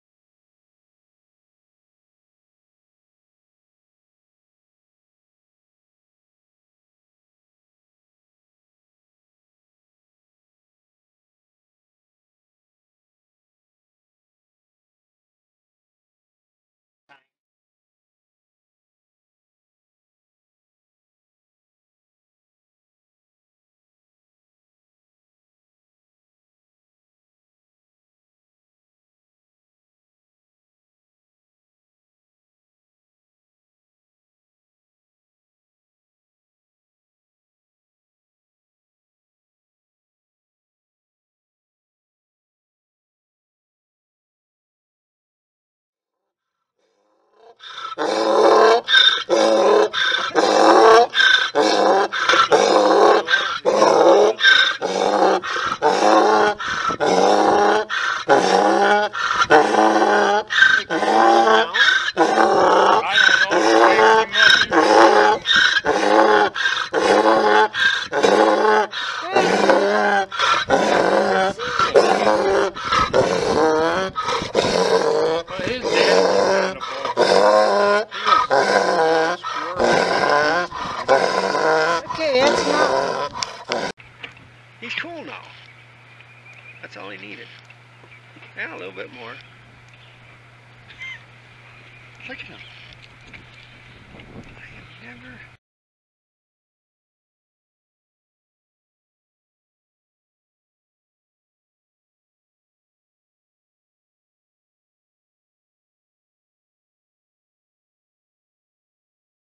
Donkeys and Farriers
Farriers Dan and Danny Tesar came to trim our equine.  Donkeys and mules.  Thor one of our Jack Stock put on a show at the end of the day after being trimmed.  It seems like he likes to calm himself down by holding onto a hoof.
You have to be there to believe it.